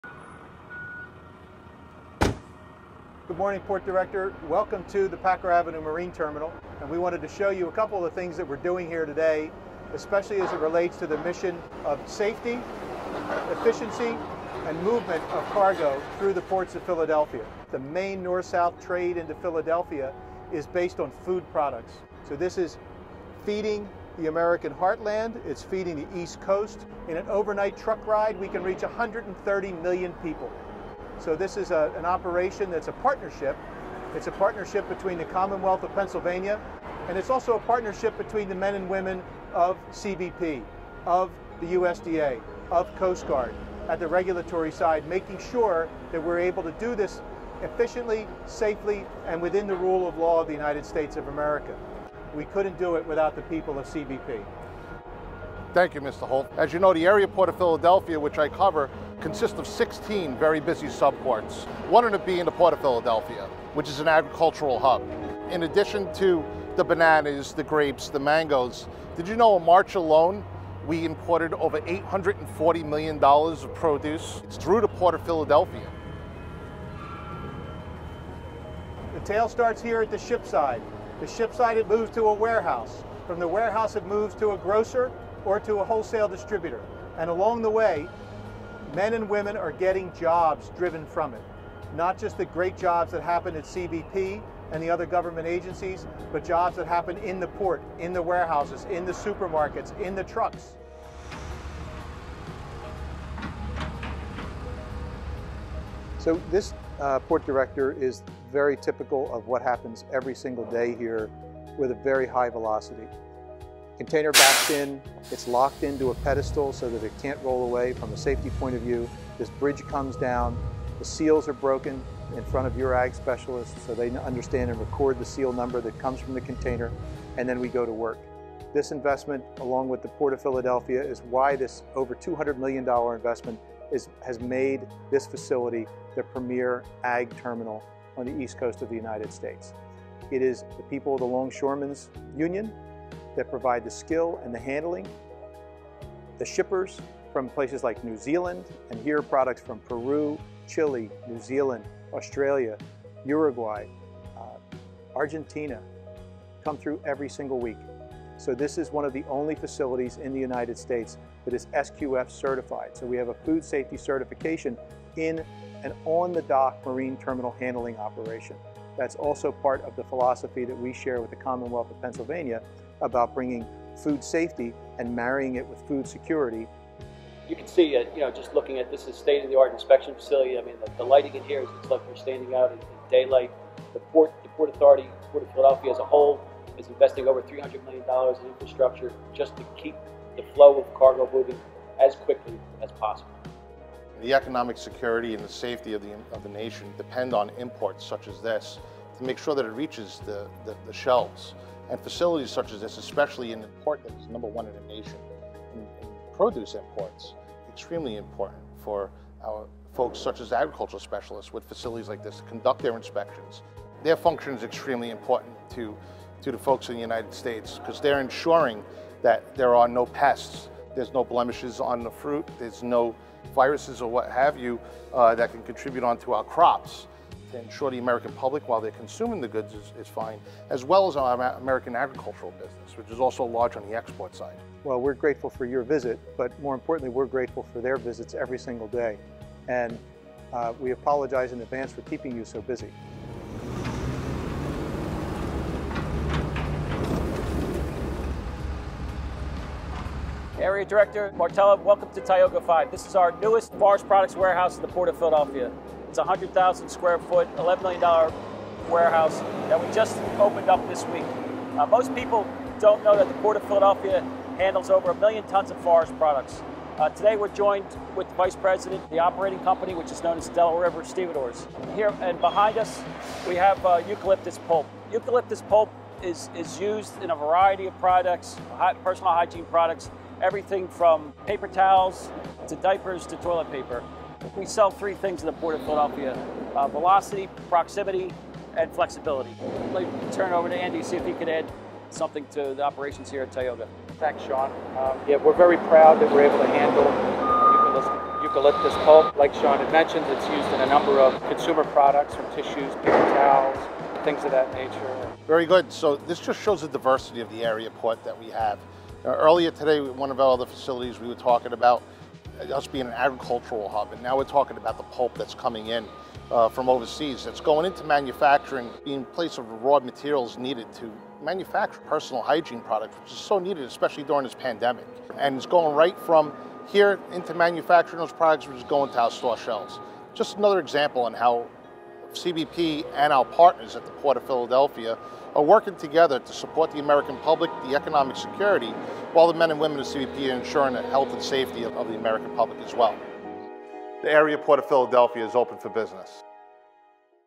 The Port of Philadelphia is Open for Business
Thanks to U.S. Customs and Border Protection - Philadelphia, our terminal operators, our dedicated longshore workers, and the broader port community, who work together every day to move cargo, keep our Nation’s store shelves full, and keep us safe → PhilaPort has remained, Open4Biz.

Video credit: U.S. Customs and Border Protection